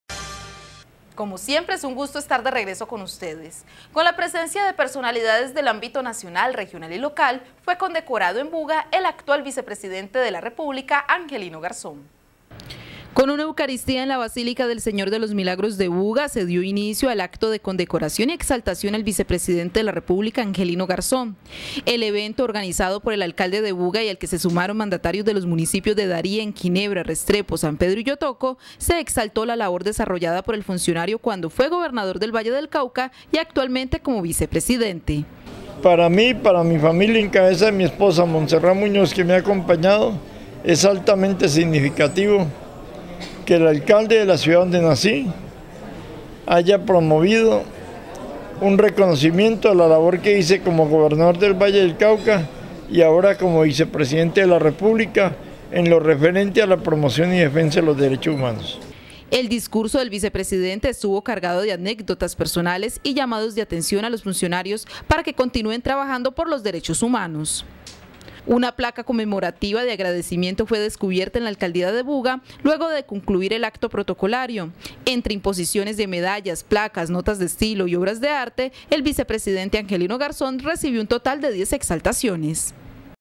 Condecoración Vicepresidente de la Republica Angelino Garzón
Con la presencia de personalidades del ámbito nacional, regional y local fue condecorado en Buga el actual Vicepresidente Angelino Garzón.